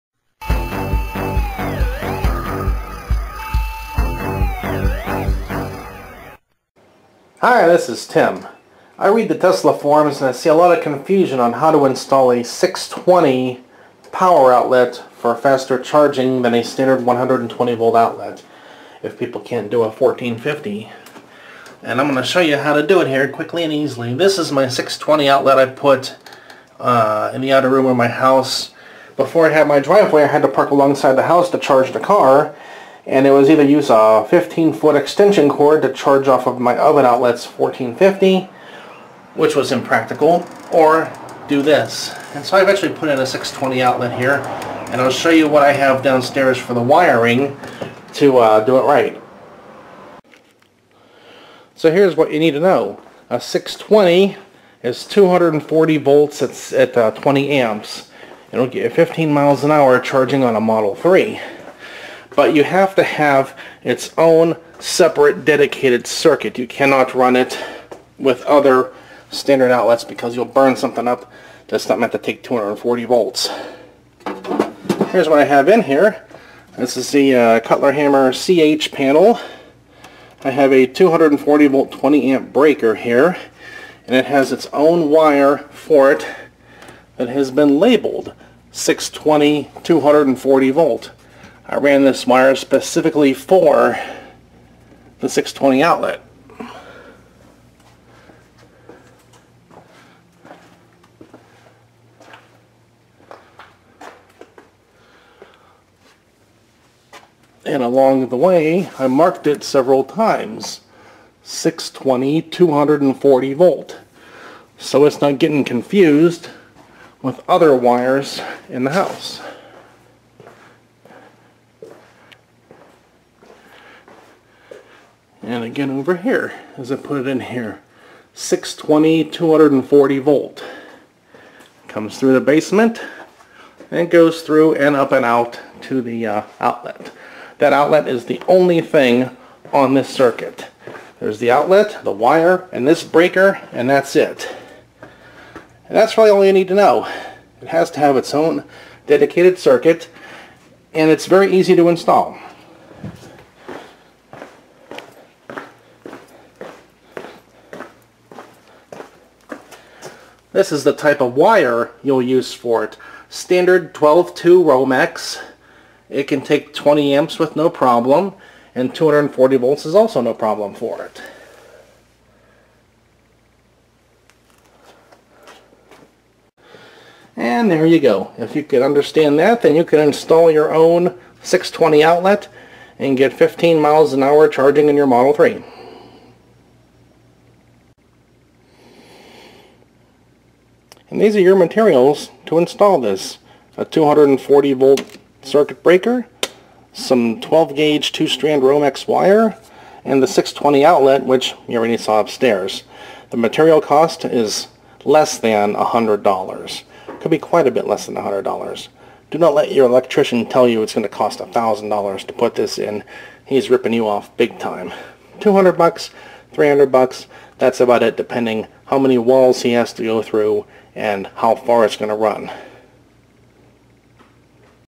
Tesla NEMA 6-20 home charging installation, fast, simple, and complete directions!
Need more speed for your Tesla charging?  Want to go 240 volt?  This video shows all the facts in a plain, simple, and brief manner!  :)